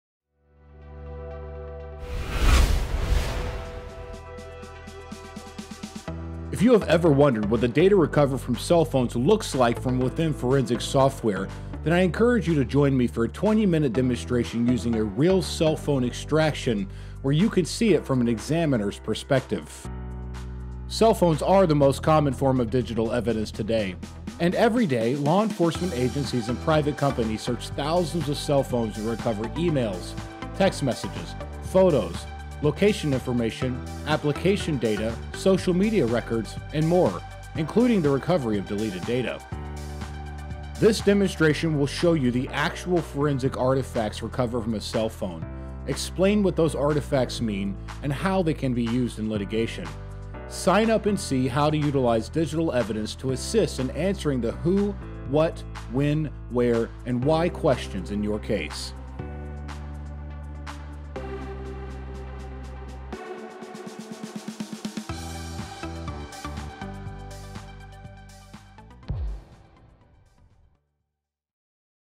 Forensic Imaging Cell Phone Demonstration
With cell phones being the most common source of digital evidence today, the need for cell phone forensics examiners is more prevalent than ever. Our digital forensics team can recover emails, text messages, photos and media, location information, application data, social media records, and even the recovery of deleted data.

Want a quick, 20-minute, live demonstration of a cell phone extraction and learn about how we go about our process? Fill out a contact form and one of our examiners will contact you.